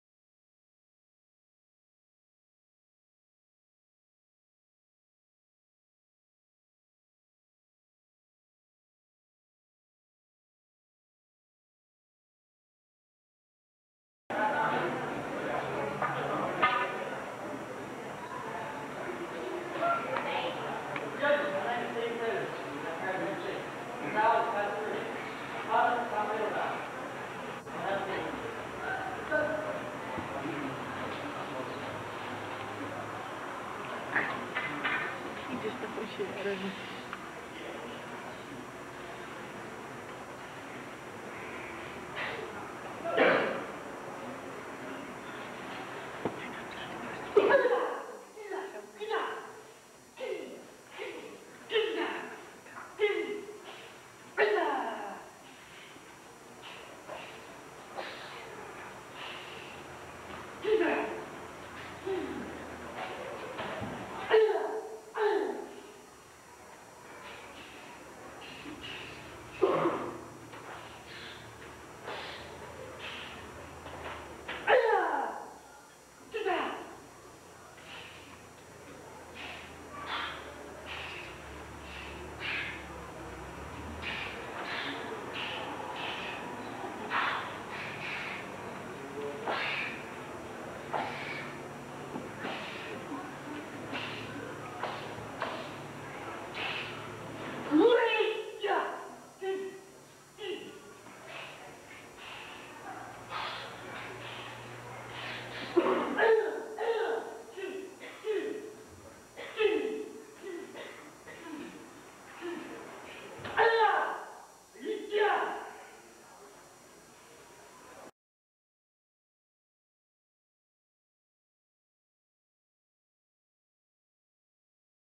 1986 David Harris Kama
1986 School Championships weapons competition. David Harris performing Kama.